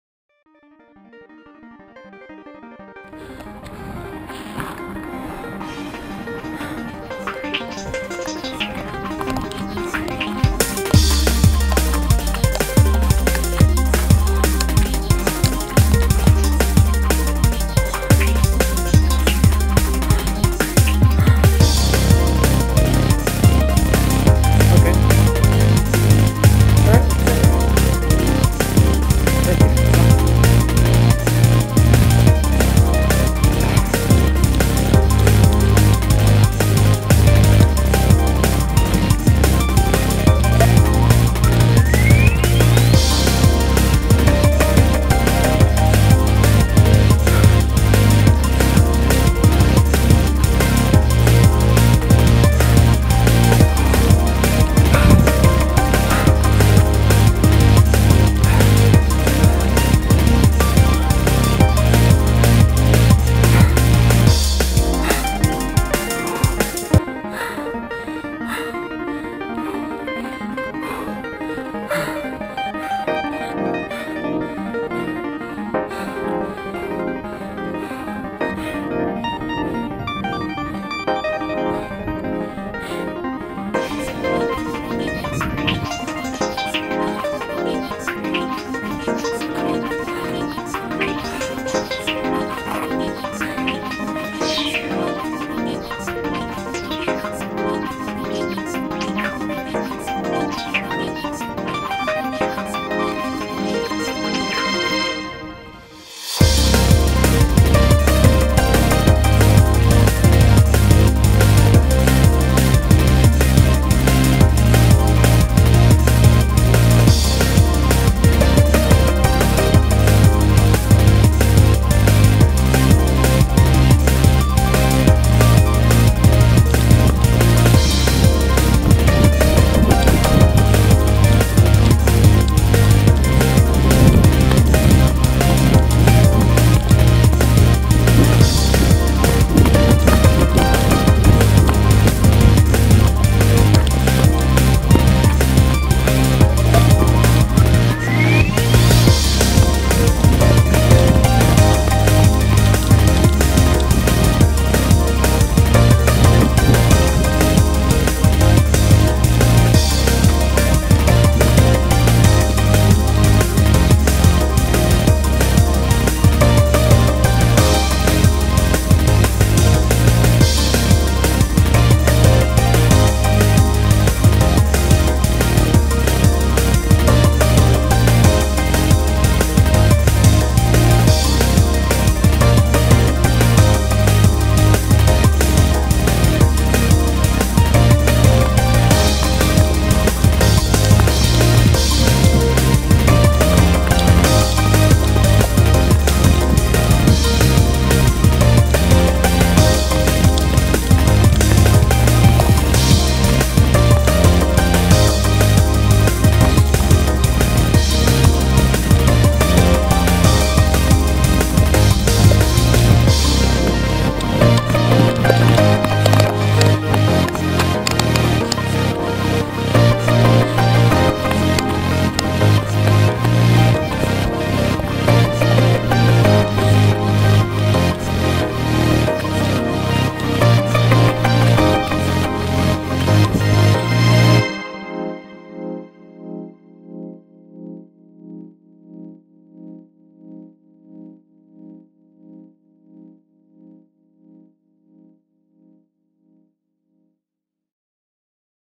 Sugar Loaf Mountain - Trail Running
I would run this trail often during my bishop high sierra ultramarathon training. GoPro Hero2